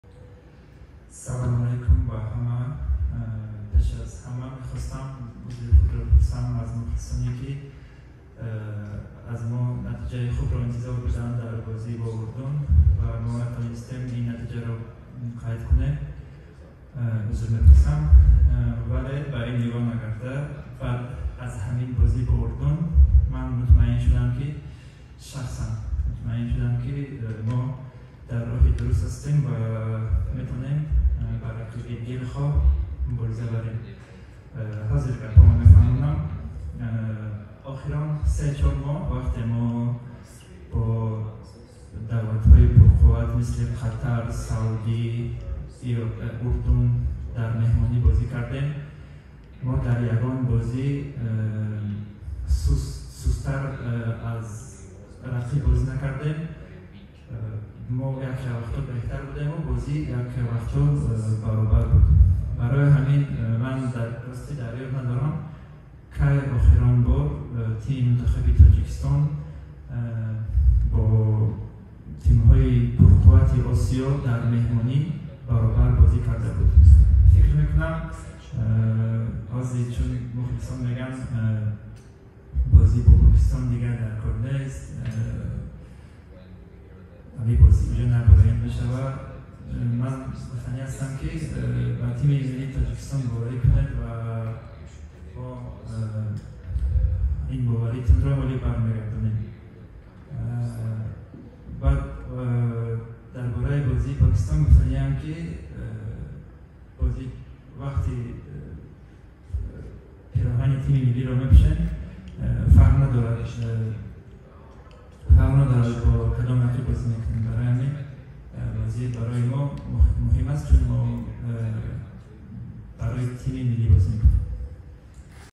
ЗОИР ҶӮРАБОЕВ: «ДАСТГИРИИ МУХЛИСОН БАРОИ ТИМИ МИЛЛИИ ТОҶИКИСТОН ХЕЛЕ МУҲИМ АСТ»
Ҳимоятгари мунтахаби Тоҷикистон Зоир Ҷӯрабоев дар бораи рӯҳияи даста пеш аз бозии ниҳоии даври дуюми марҳилаи интихобии Ҷоми ҷаҳон-2026 дар гурӯҳи «G» бо тими миллии Покистон сухан кард. Ин вохӯрӣ рӯзи 11 июн дар Варзишгоҳи марказии Душанбе баргузор шуда, соати 20:00 оғоз мешавад.

Зоир ҶӮРАБОЕВ, ҳимоятгари тими миллии Тоҷикистон:

– Пеш аз ҳама аз мухлисонамон, ки дар бозии сафарӣ бо тими миллии Урдун аз мо натиҷаи хуб интизор буданд, узр мехоҳам. Мутаассифона, мо интизориҳои онҳоро бароварда натавонистем. Бо вуҷуди ин, мехоҳам ба мухлисон бигӯям, ки дастаи мо дар роҳи дуруст аст. Чаро ман инро мегӯям? Дар Ҷоми Осиё-2023 ва марҳилаи интихобии Ҷоми ҷаҳон-2026 бо тимҳои пурқуввате чун Қатар, Урдун ва Арабистони Саудӣ бозӣ кардем. Ва дар бозиҳои сафарӣ тими мо заифтар набуд, танҳо рақибон аз баъзе хатоҳои хурди мо истифода карданду мо аз имкониятҳои худ истифода набурдем. Ҳоло дар шабакаҳои иҷтимоӣ бархе аз мухлисон менависанд, ки вохӯрӣ бо Покистон ҷолиб нест. Ман аз мухлисон хоҳиш дорам, ки дар чунин лаҳзаи душвор тими моро дастгирӣ кунанд. Умедворам, ки боз боварии мухлисонро пайдо мекунем ва собит мекунем, ки дар тими мунтахаби кишвар ба хотири ватан ҳунарнамоӣ мекунем.

ЗОИР ДЖУРАБОЕВ: «СБОРНОЙ ТАДЖИКИСТАНА ОЧЕНЬ ВАЖНА ПОДДЕРЖКА БОЛЕЛЬЩИКОВ»

Защитник национальной сборной Таджикистана Зоир Джурабоев рассказал о том, с каким настроем команда подходит к матчу заключительного тура второго раунда отборочного турнира чемпионата мира-2026 в группе «G» против сборной Пакистана. Встреча пройдет во вторник, 11 июня, на Центральном стадионе Душанбе и начнется в 20:00 часов.

Зоир ДЖУРАБОЕВ, защитник национальной сборной Таджикистана:

– Прежде всего, хочу принести извинения нашим болельщикам, которые ожидали хороший результат от нас в гостевом матче со сборной Иордании. К большому сожалению, мы не смогли оправдать их ожидания. Однако хочу сказать болельщикам, что наша сборная на правильном пути. Почему я так говорю? На Кубке Азии-2023 и в отборочном турнире чемпионата мира-2026 мы играли против таких сильных сборных, как Катар, Иордания и Саудовская Аравии. И в выездных матчах по игре наша сборная не была слабее, просто соперники пользуются какими-то нашими мелкими ошибками, а мы не используем свои моменты. У меня огромная просьба к болельщикам, чтобы они поддержали нашу сборную в столь трудный момент. Надеюсь, что мы вернем доверие болельщиков и докажем, что играем за национальную сборную не просто так, а ради страны.